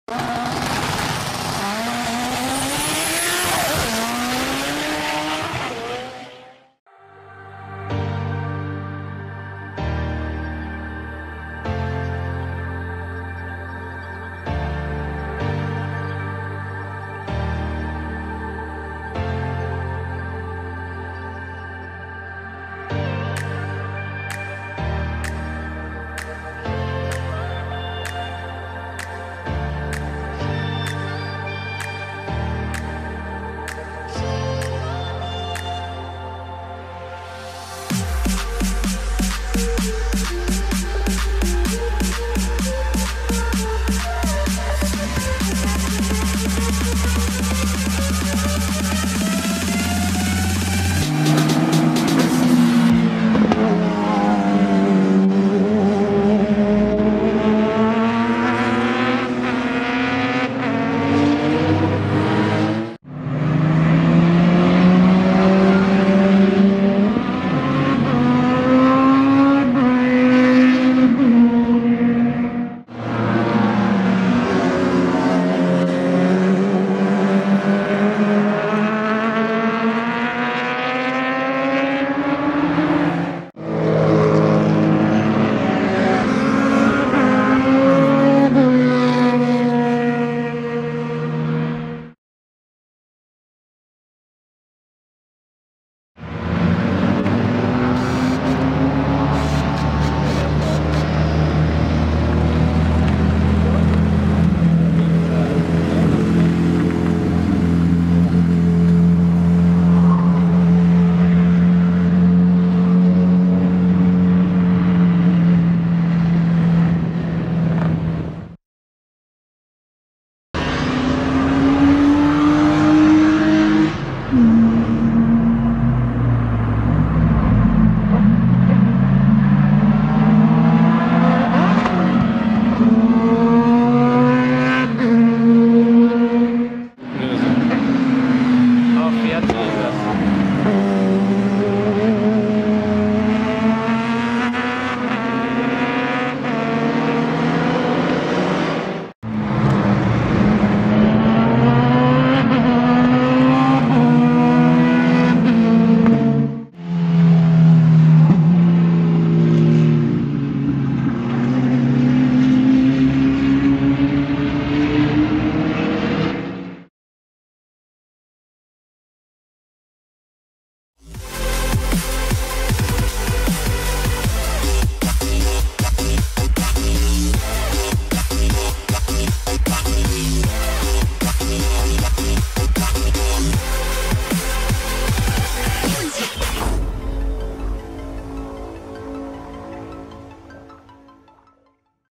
Fiat 126 F2 Widebody with Yamaha R1 Engine 180HP @ Circuit Zolder Belgium
Fiat 126 F2 Widebody with Yamaha R1 Engine Swap 180HP @ Circuit Zolder Belgium

We spotted a cool widebody car, filmed it and asked the owner after the trackday about the car.

This car is a FIAT 126 F2 Widebody with an engine swap of a Yamaha R1 bike.
The sound of this car was the only one that stood out among the other cars.

This video was filmed with a Canon 700D & Canon PowerShot SX740 HS.

Best Regards

Kimbo